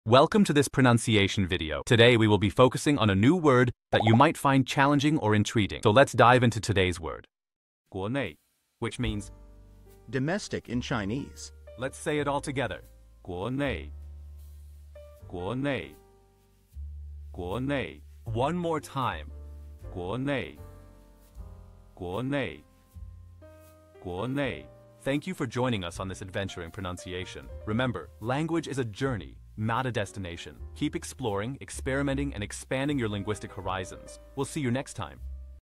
How to pronounce guó nèi | 国内 (domestic in Chinese)
Master the Pronunciation of 'guó nèi | 国内' - which means : domestic in Chinese 📝 with @PerfectPronunciationChinese 🎓🗣️ - [Your Guide to Chinese]

Learn to pronounce 'guó nèi' perfectly in Chinese with our easy and engaging tutorial by @PerfectPronunciationChinese! 📖📽️ In this comprehensive video, we provide you with native speaker examples 🗣️👂 and phonetic breakdowns 🎵 to ensure you get the pronunciation just right.